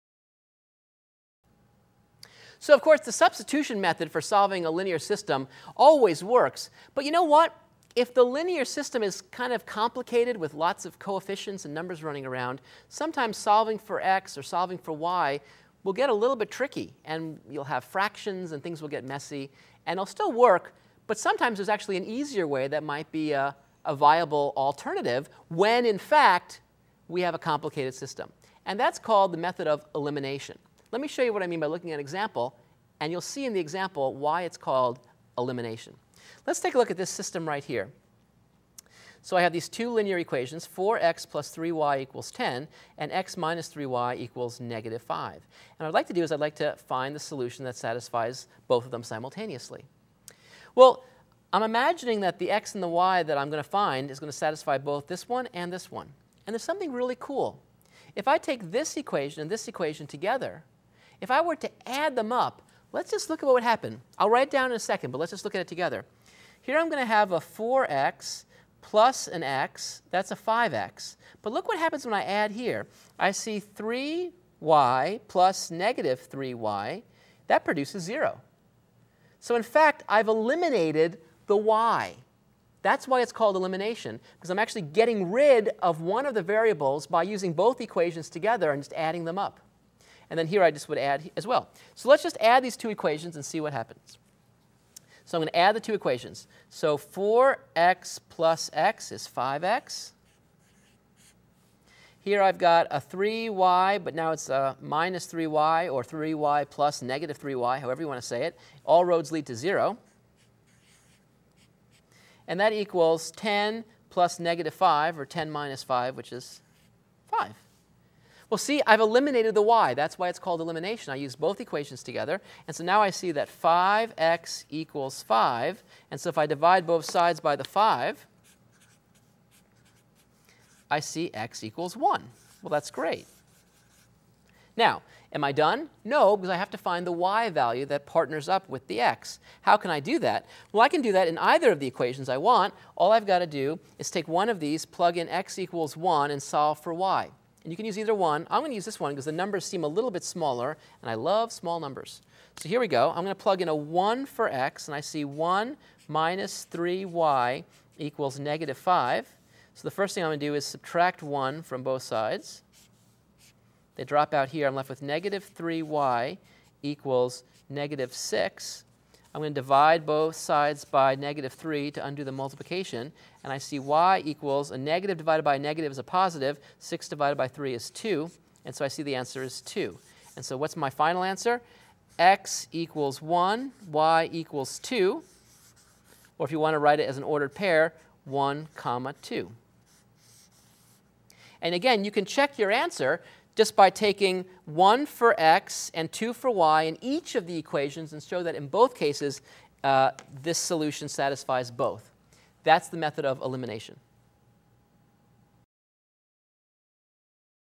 The Elimination Method: Adding
Chapter 7 Systems of Equations and Inequalities, Subchapter 7.1 Solving Systems of Two Linear Equations in Two Variables